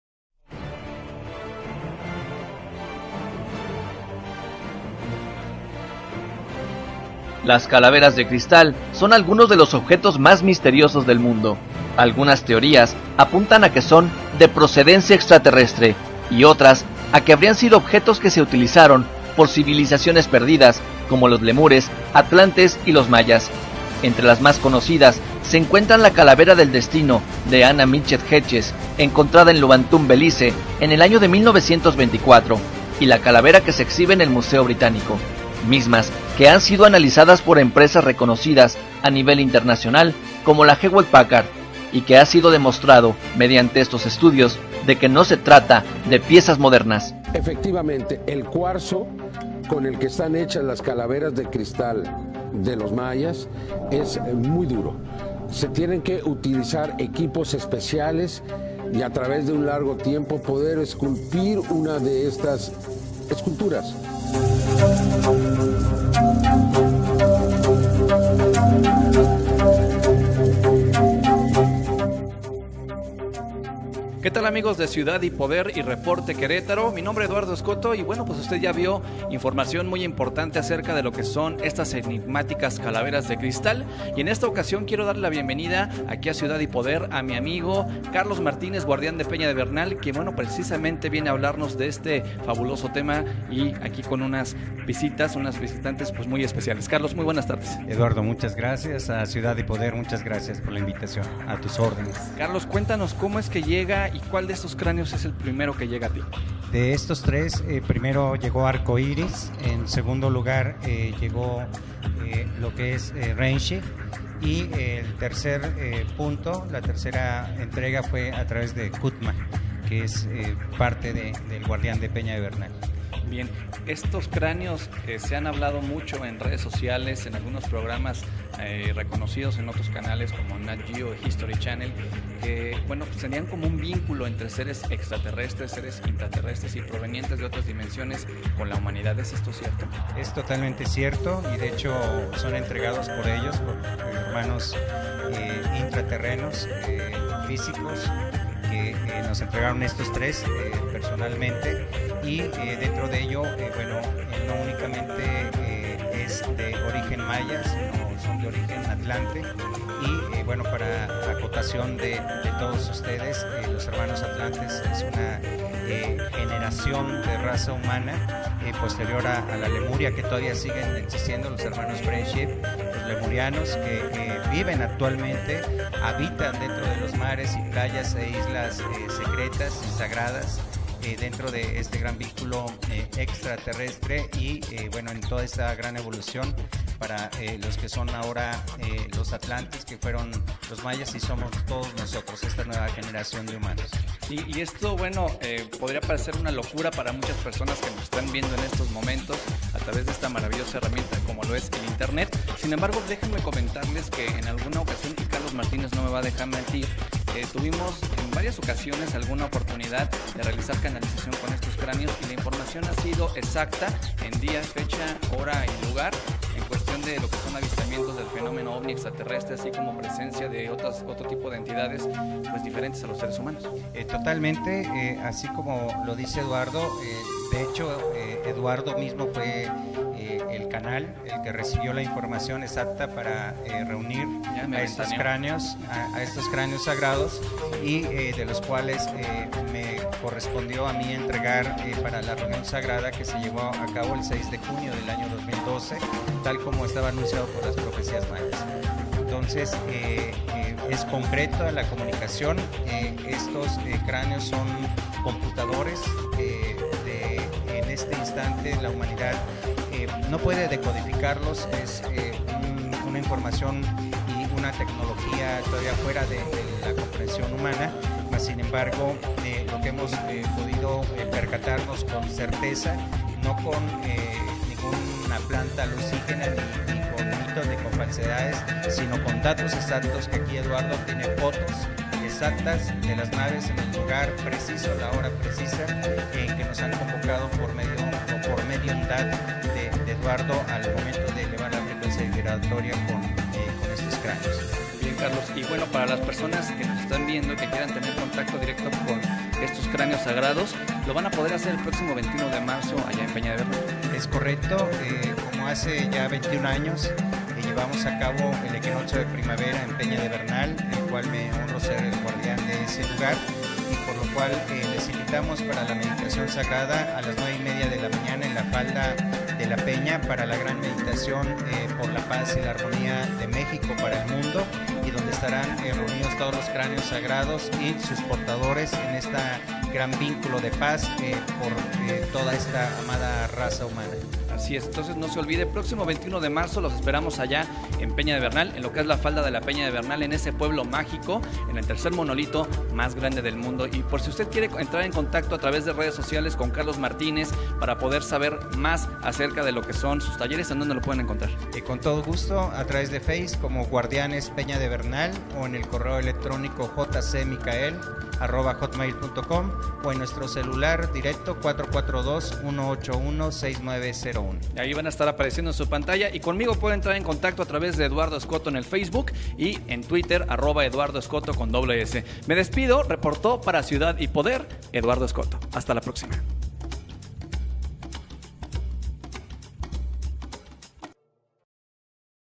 ARCHIVO INSÓLITO: CALAVERAS DE CRISTAL EN BERNAL
Este 21 de Marzo en la Peña de Bernal en el estado de Querétaro se llevará acabo una ceremonia y meditación con Calaveras de Cristal y sus portadores. ¿ Que son las Calaveras de Cristal? Aquí le compartimos la información y entrevista con Carlos Martínez Guardián de la Peña de Bernal.

Los Esperamos en Peña de Bernal Sábado 21 de Marzo 2015 en la falda de la Peña.

Evento totalmente gratuito, vestir de Blanco con pañuelo o marcada Violeta o rojo.
Llegar al lugar a las 9:30 am. ( Prever que habrá más de 25 000 Personas en el Pueblo, por lo que consideres llegar mucho más temprano).
Traer sombrero.
Tiempo de Meditación de 9:30 am a 1:00 pm.
Toda la Meditación estará acompañada del "Consejo Cristal Arcoiris de Portadores de Cráneo Sagrado".